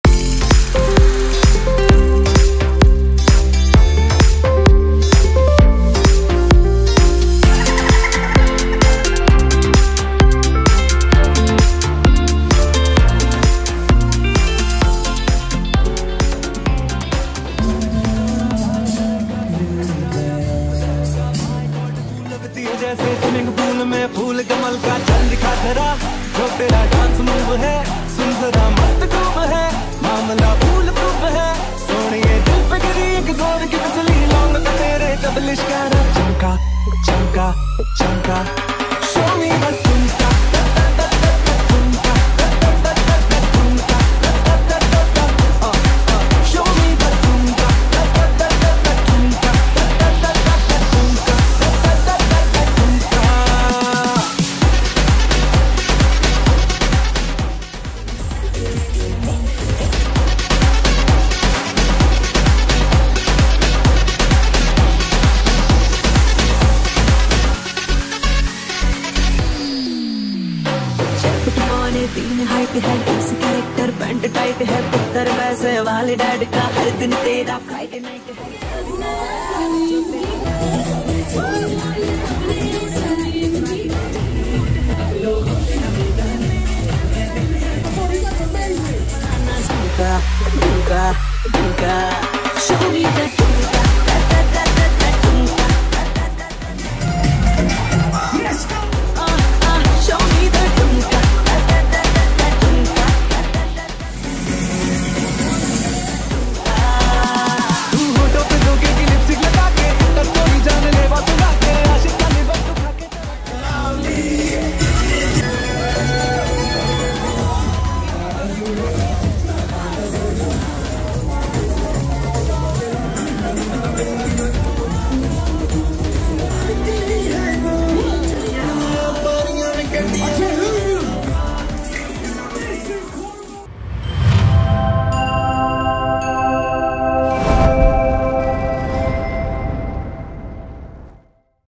Show me your Thumka Party
Instant Karma Bollywood Concerts - Parties - Events for bringing an all women comedy evening , such a fun, full of laughter evening, followed by show me thumka evening with Dj AM and Roneel Prakash
Shalini Ramachandran got Hidden  Talent
Shaikh Mona got Best of the Best  jokes
Congratulations Reema Khanolkar for launch of your musical

Videography by KingStudio